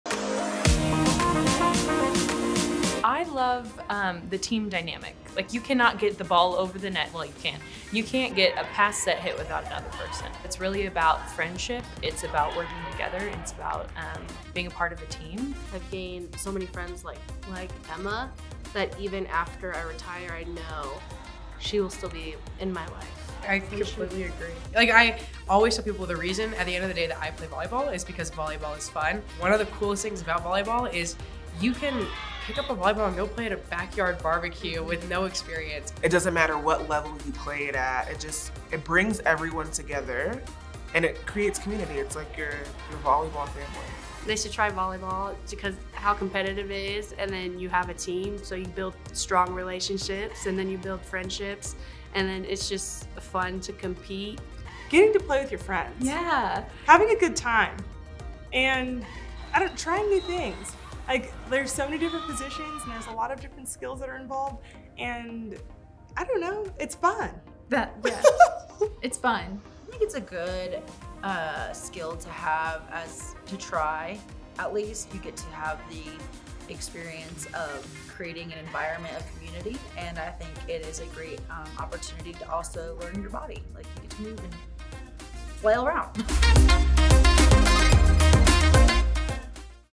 U.S. Women's Sitting National Team | What do you love about volleyball? | Try Volleyball
U.S. Women's Sitting National Team athletes talk about what they love about volleyball.

Also listen to (or watch) the latest episodes of The USA Volleyball Show now! Find new episodes wherever you get your podcasts!